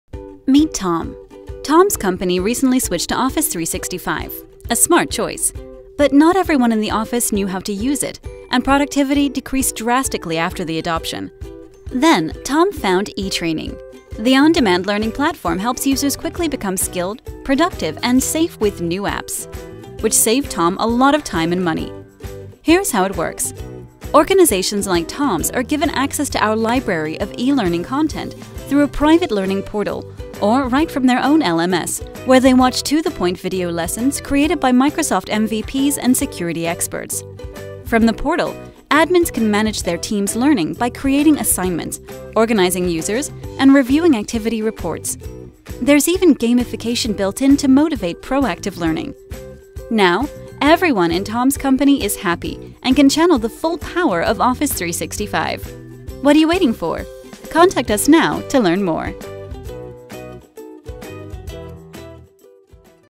Reality Bytes eTraining - Learning Management System Overview
60 second walkthrough on the eTraining system available through Reality Bytes